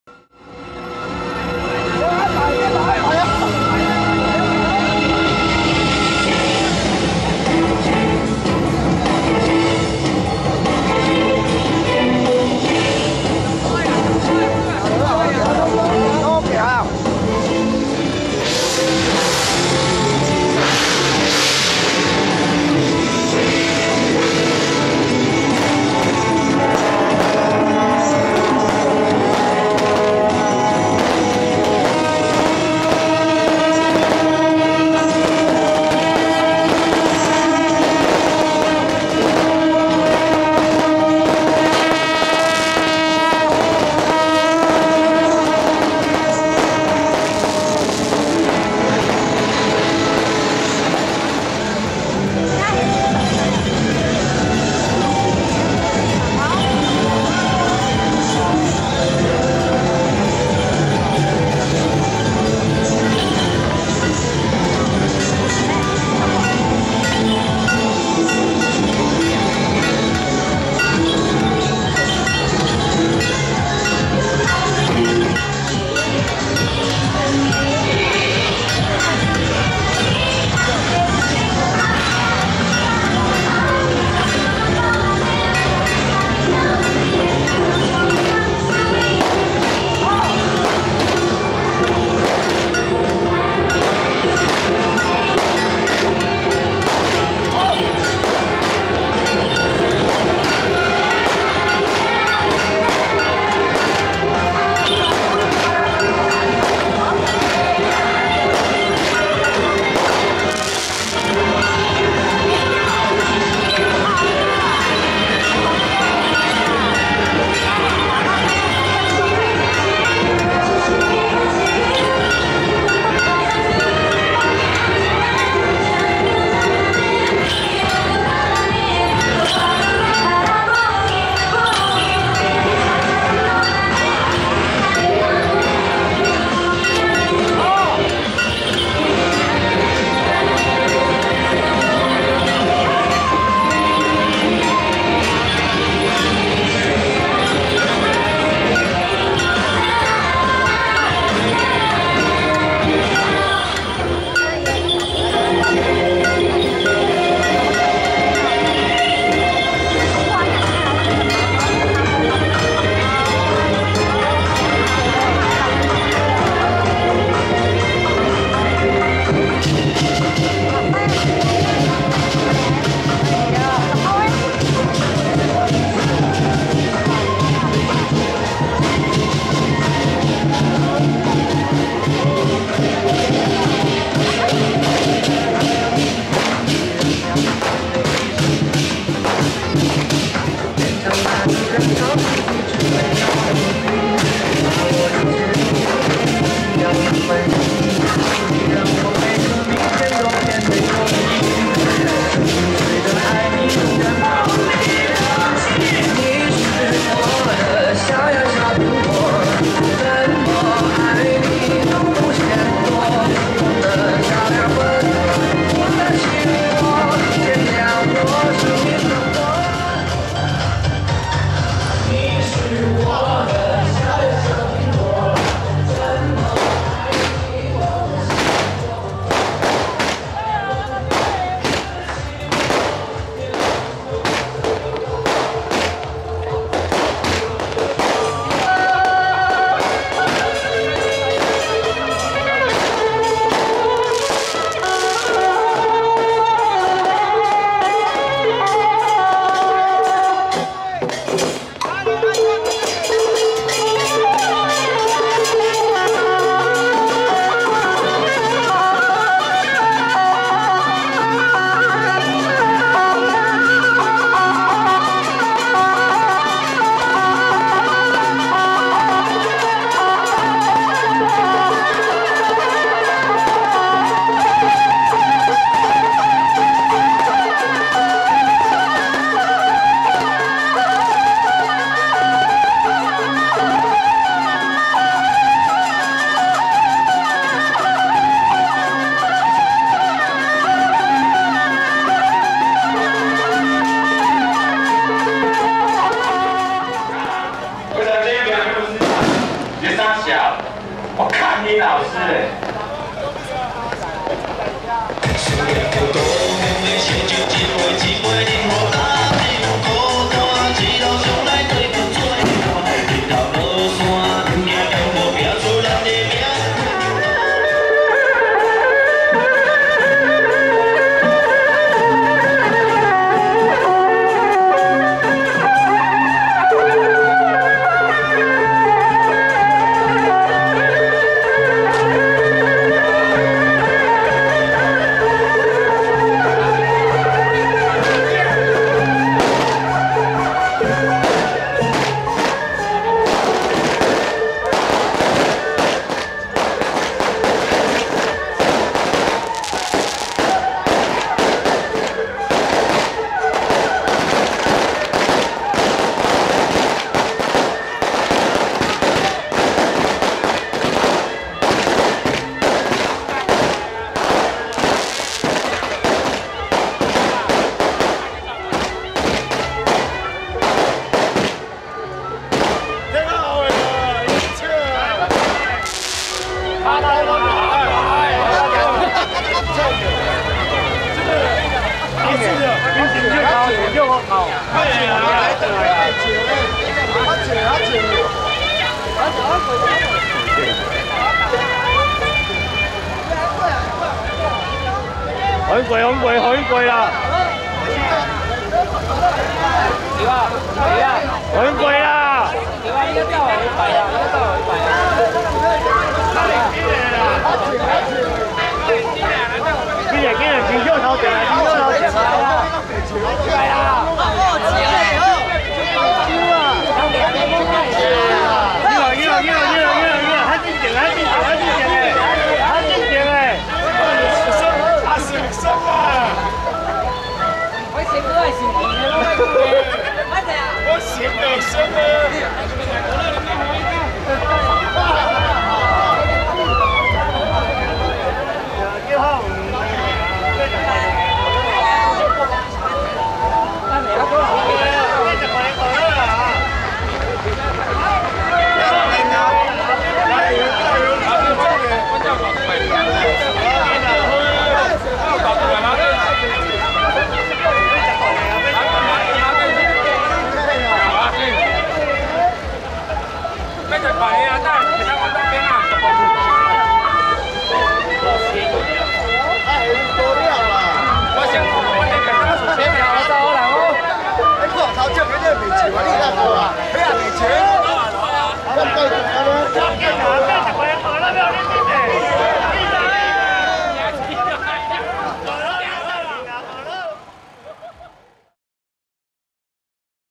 屏東縣崁頂鄉~北極宮~重建廟三十三週年慶平安繞境~05
104年01月03日(六)屏東縣崁頂鄉北極宮玄天上帝歲次甲午年重建廟三十三週年慶平安繞境~05